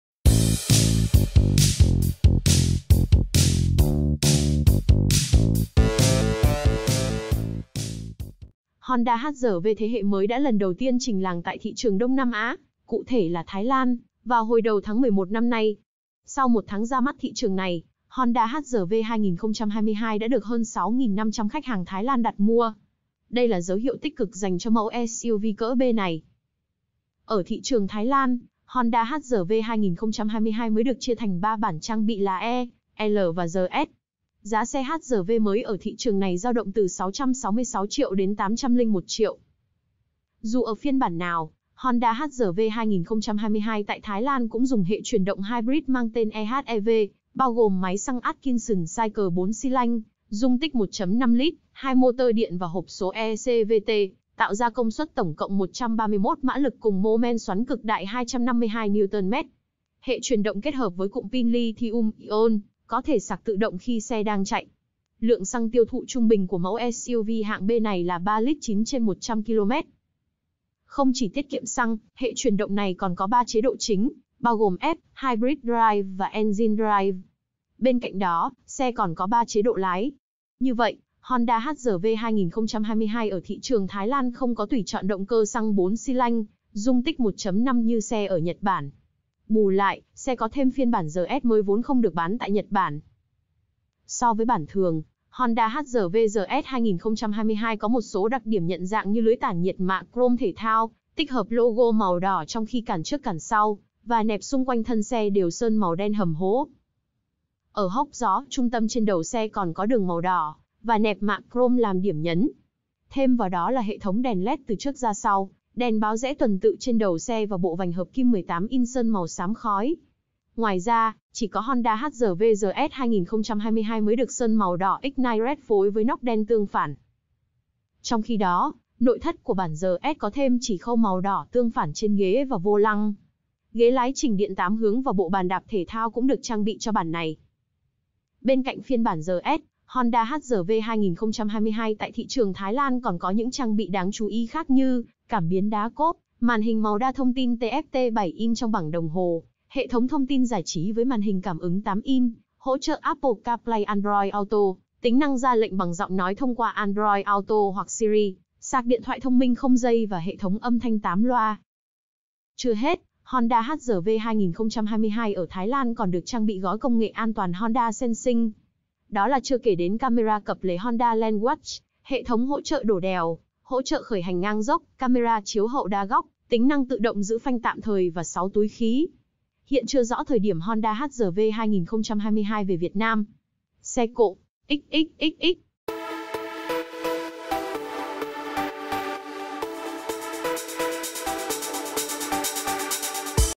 Honda HR-V 2022 giá từ 666 triệu chỉ tốn 3.7L/100Km được người dùng đặt mua với doanh số khủng_XE CỘ
Sau gần 1 tháng ra mắt tại Thái Lan, Honda HR-V 2022 đã được hơn 6.500 khách hàng đặt mua. Đây được xem là doanh số ấn tượng so với các đối thủ cùng phân khúc.